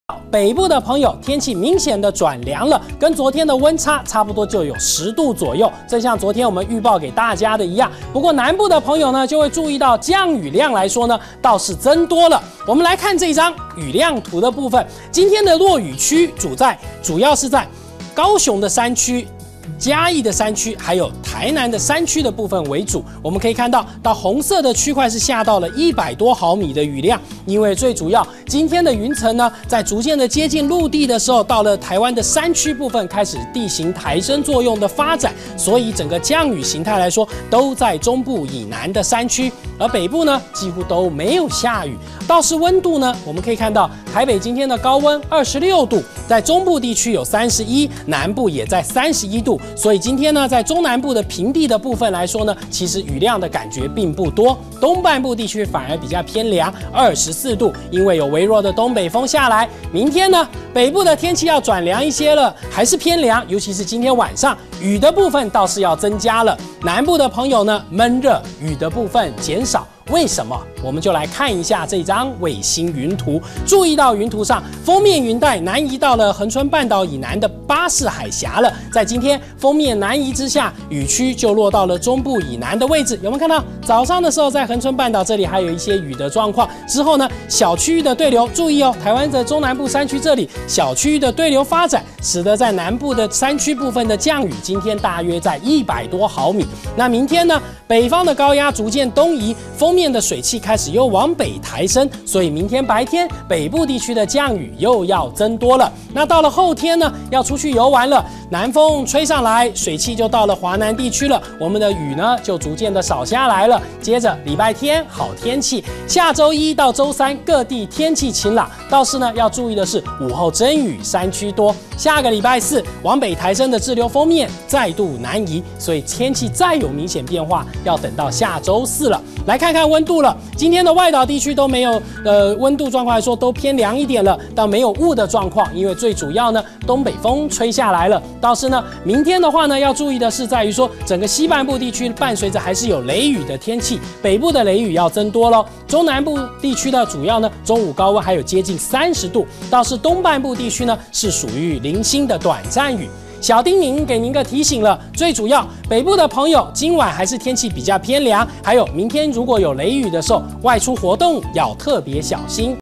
【中天】5/3 戴立綱報氣象
今天南部山區降雨多，明天雨區北移，雷陣雨山區居多，戶外活動要小心！週一起將有午後陣雨。小叮嚀：局部陣雨明顯、雷雨時要特別小心。